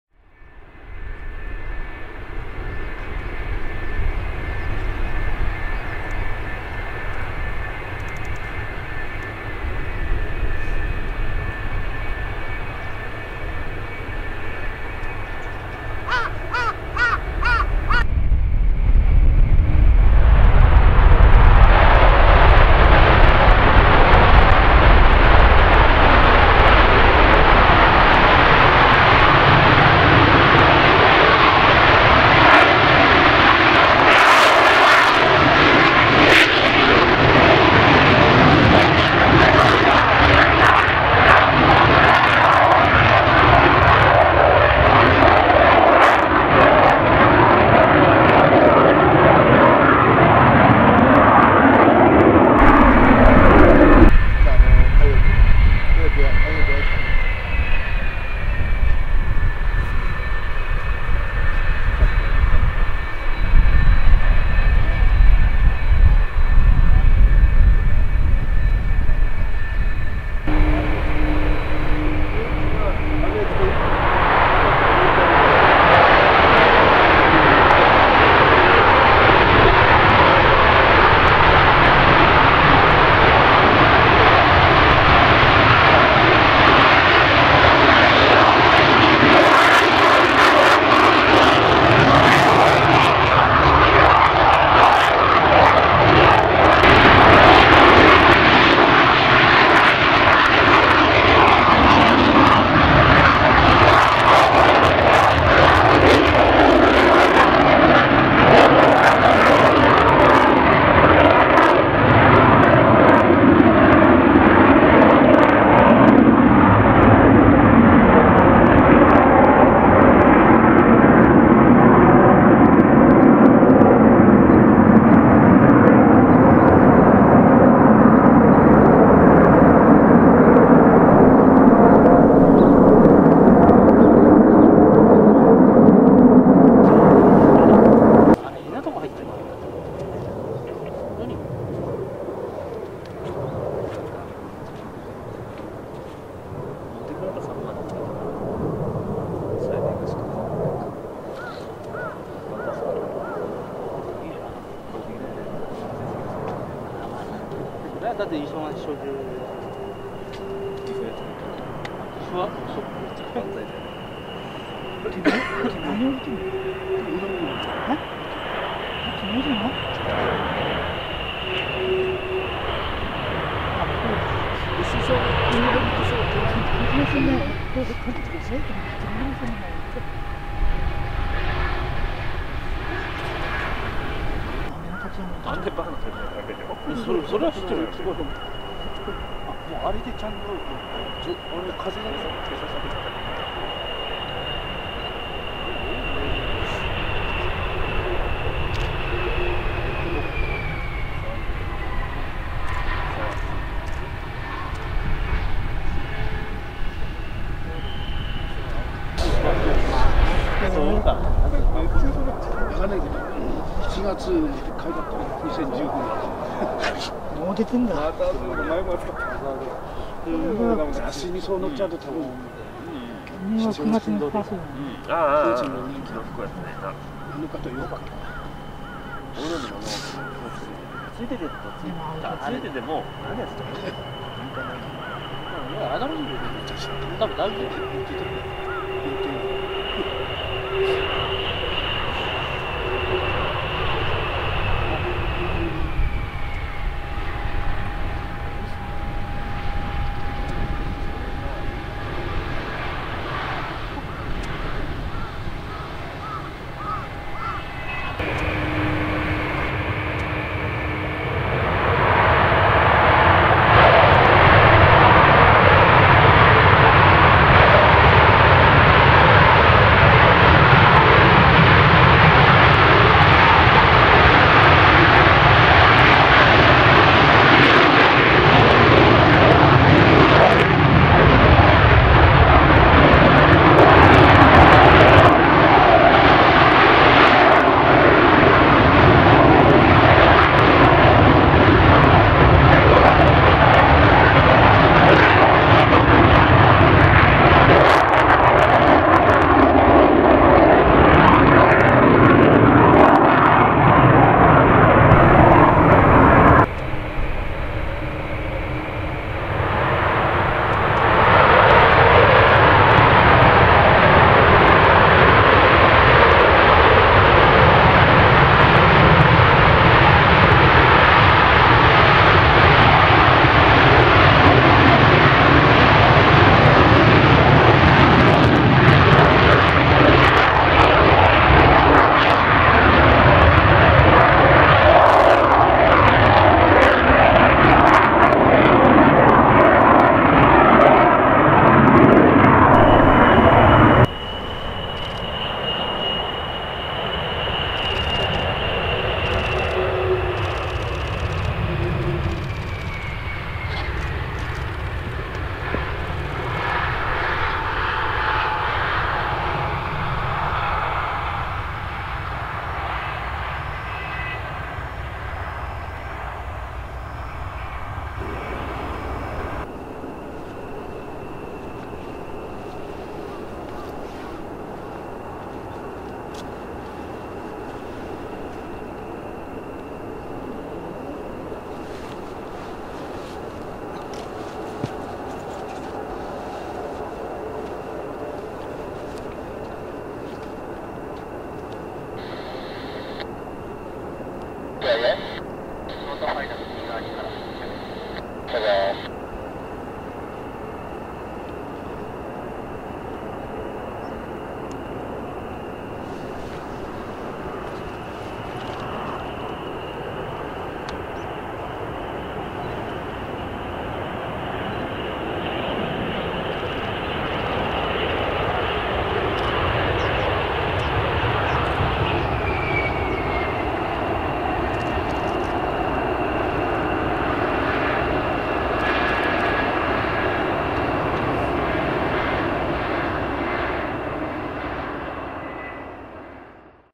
✈2014/11/27 501SQ RF4E/EJ landing & takeoff @Hyakuri Air Base(茨城空港/百里基地)
茨城空港ファントム広場にて。
■movie camera : JVC Everio Gz-E565
■IC recorder : sony ICD-UX523
■microphone : Audio technica AT9913